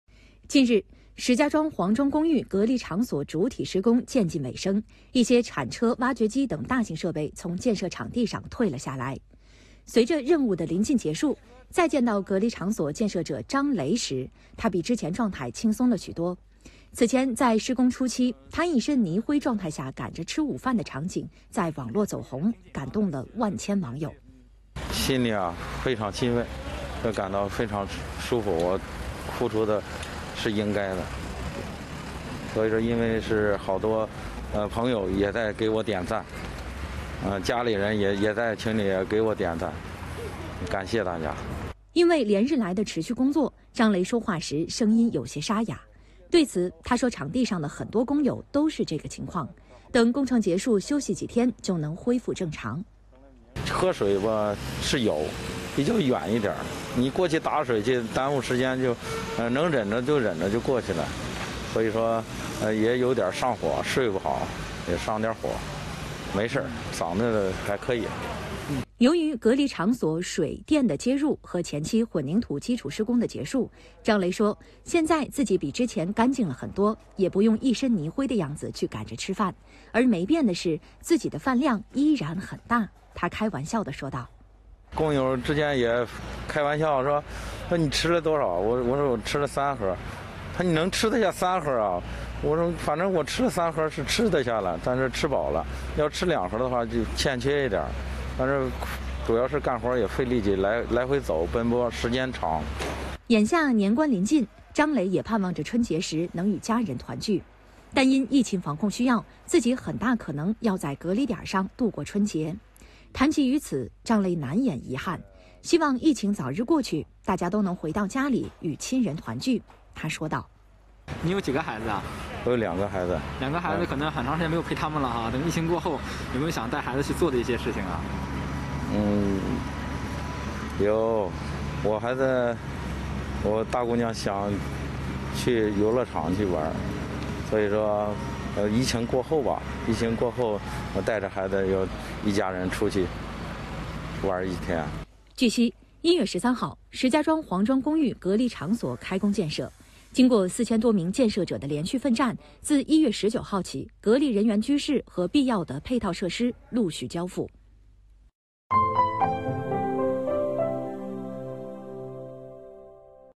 满身灰泥受网友点赞 石家庄隔离场所建设者张雷：付出是应该的
近日，石家庄黄庄公寓隔离场所主体施工渐近尾声，一些铲车、挖掘机等大型设备从建设场地上退了下来。
        随着任务的临近结束，再见到隔离场所建设者张雷时，他比之前状态轻松了许多。此前在施工初期，他一身泥灰状态下赶着吃午饭的场景在网络走红，感动了万千网友。
        据悉，1月13日，石家庄黄庄公寓隔离场所开工建设。经过4000多名建设者的连续奋战，自1月19日起，隔离人员居室和必要的配套设施陆续交付。